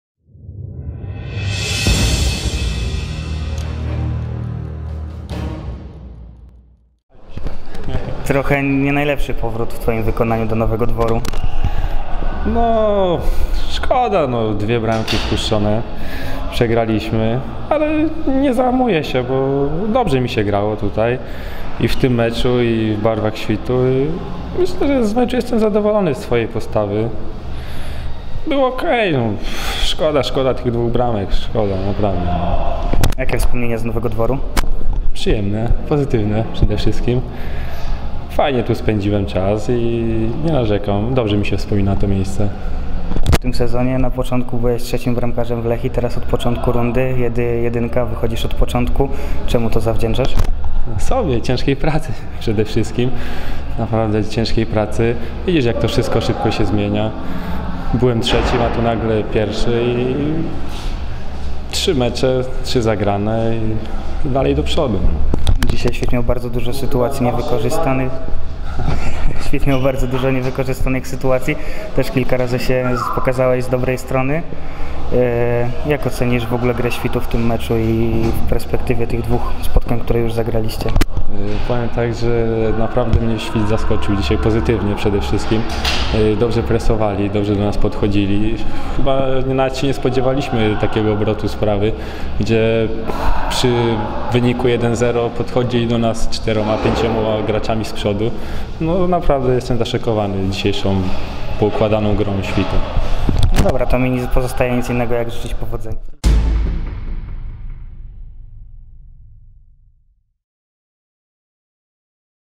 Kędra: Dobrze mi się grało w barwach Świtu. Świt - Lechia 2:0 25.03.2017
Wypowiedź bramkarza Lechii Tomaszów Mazowiecki - Dawida Kędry po meczu 20. kolejki III ligi:
MKS Świt Nowy Dwór Mazowiecki - Lechia Tomaszów Mazowiecki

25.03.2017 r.

Rozmawiał: Michał Kulesza
Zdjęcia: Rafał Gadzała
Montaż: Adam Szabłowski

2017